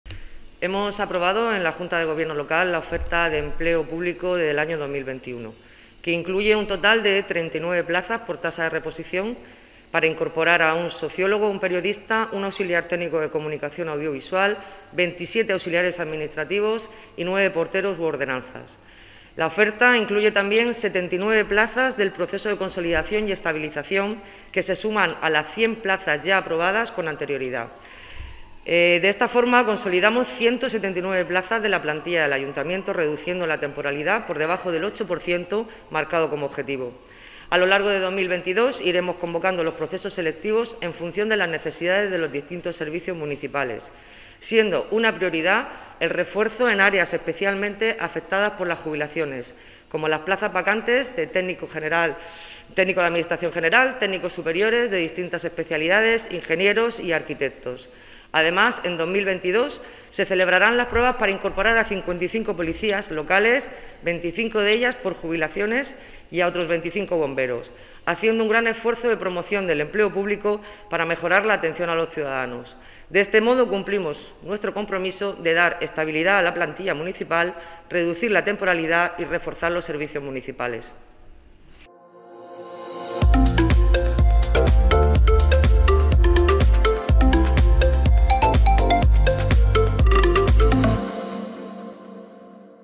La Junta de Gobierno aprueba la Oferta de Empleo Público de 2021
La Junta de Gobierno ha aprobado la Oferta de Empleo de 2021, que incluye un total de 39 nuevas plazas por tasa de reposición para incorporar a un sociólogo, un periodista, un auxiliar técnico de comunicación audiovisual, 27 auxiliares administrativos y 9 porteros u ordenanzas.

La oferta incluye también 79 plazas del proceso de consolidación y estabilización donde hay que destacar las 35 de educadores de escuelas infantiles y ocho profesores que, junto con ingenieros, arquitectos y otras categorías se suman a las 100 plazas ya aprobadas con anterioridad. Lo que supone, según la concejal de Hacienda y Personal, Esperanza Nieto, que “consolidemos 179 plazas de la plantilla del Ayuntamiento, reduciendo la temporalidad por debajo del 8%, marcado como objetivo”.

Nieto añade que a estas plazas hay que añadir las 125 plazas que el Ayuntamiento tiene pendiente asignar de las ofertas de empleo de los años 2018, 2019 y 2020. De este paquete, 80 corresponden a procesos ya en marcha para incorporar a 55 policías locales, 25 de ellas por jubilaciones, y a 25 bomberos. “Hacemos un gran esfuerzo de promoción del empleo público para mejorar la atención a los ciudadanos”, ha dicho Nieto.

La edil de Personal ha asegurado que “a lo largo de 2022 iremos convocando los procesos selectivos en función de las necesidades de los distintos servicios municipales, siendo una prioridad el refuerzo de áreas especialmente afectadas por las jubilaciones, como las plazas vacantes de técnico general de administración, técnicos superiores de distintas especialidades, ingenieros y arquitectos”. De este modo, según la concejal, “cumplimos nuestro compromiso de dar estabilidad a la plantilla municipal, reducir la temporalidad y reforzar los servicios municipales”.